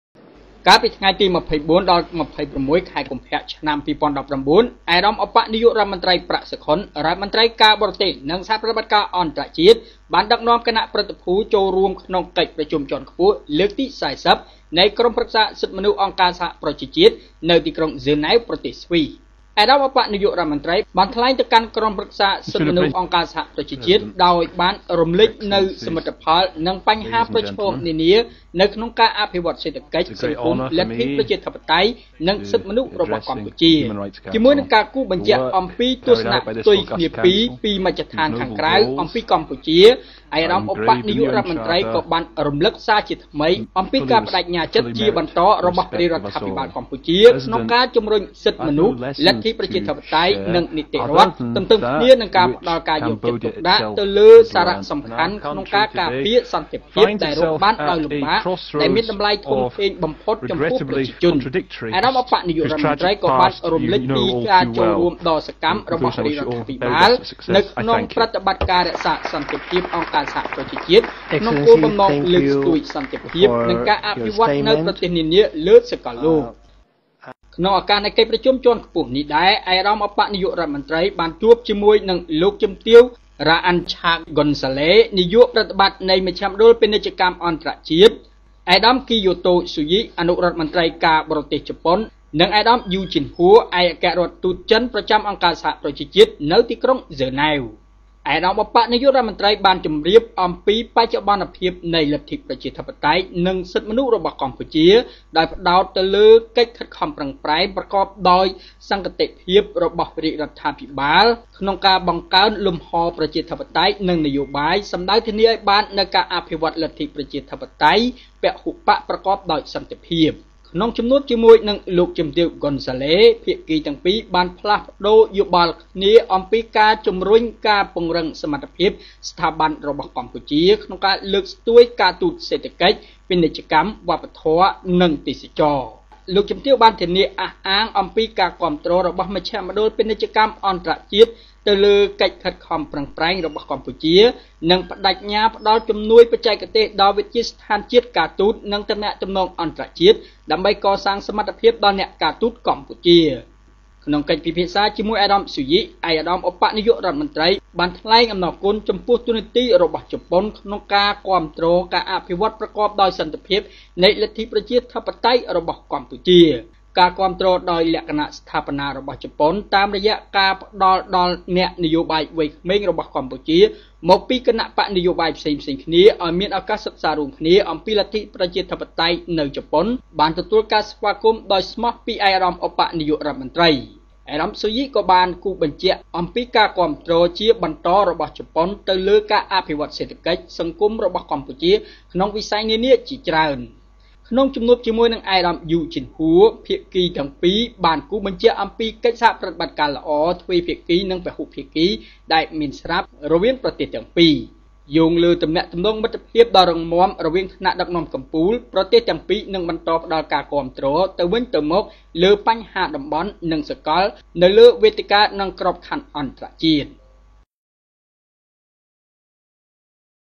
Outcomes of the participation of H.E. Deputy Prime Minister Prak Sokhonn, in Geneva.
Outcomes of the participation of H.E. Deputy Prime Minister Prak Sokhonn at the High Level Segment of the 40th Session of Human Rights Council and bilateral meetings with foreign dignitaries, held on 25 February 2019, in UN Headquarters in Geneva.